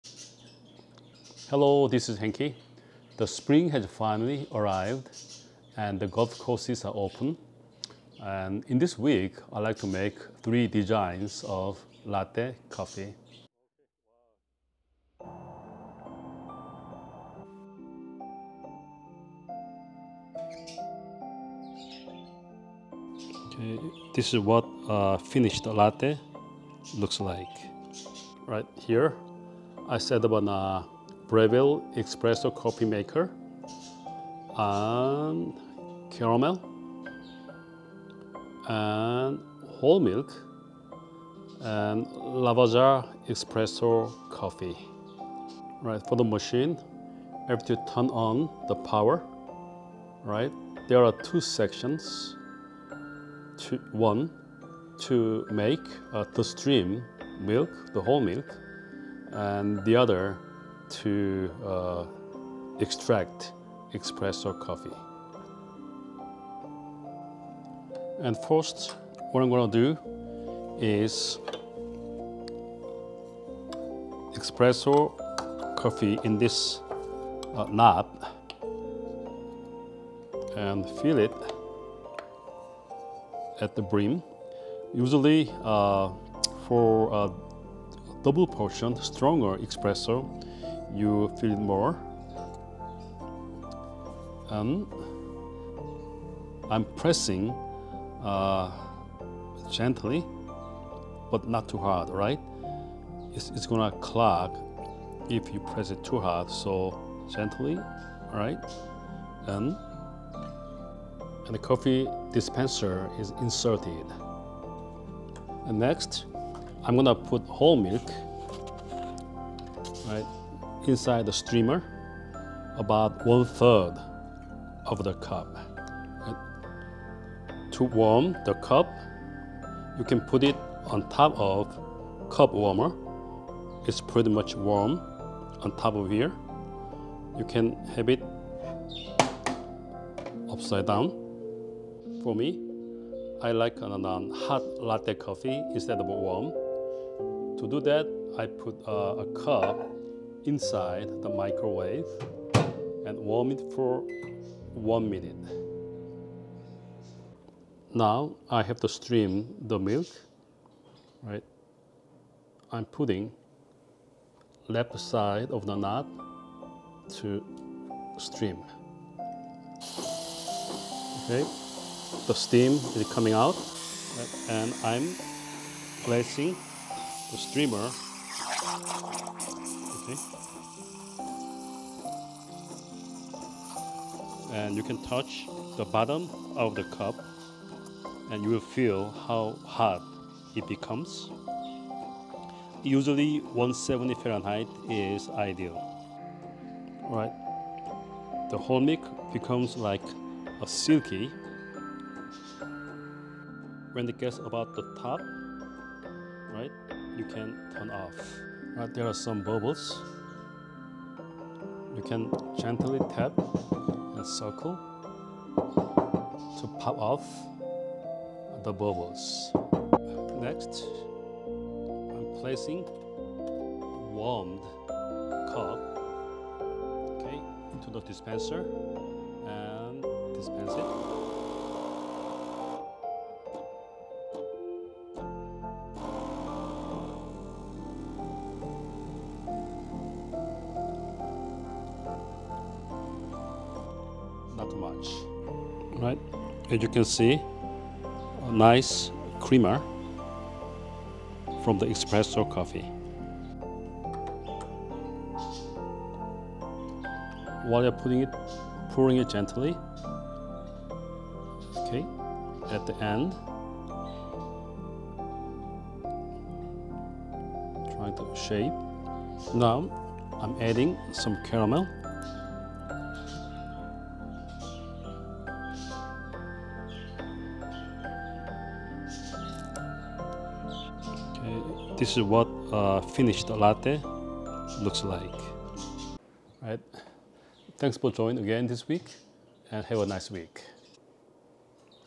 How to make latte coffee
I am showing step by step to make a hoe delicious latte coffee